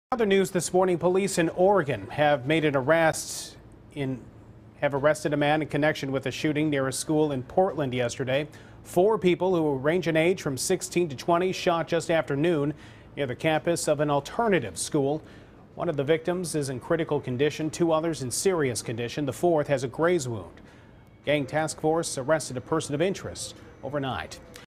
Portland, Oregon police make an arrest in school shooting.
olice in Portland, Oregon have arrested a man in connection with a shooting near a school Friday.
Four people, who range in age from 16 to 20, were shot just after noon near the campus of an alternative school.
One of the victims is in critical condition, two others in serious. the fourth has a graze wound.
A gang task force arrested a “person of interest” overnight.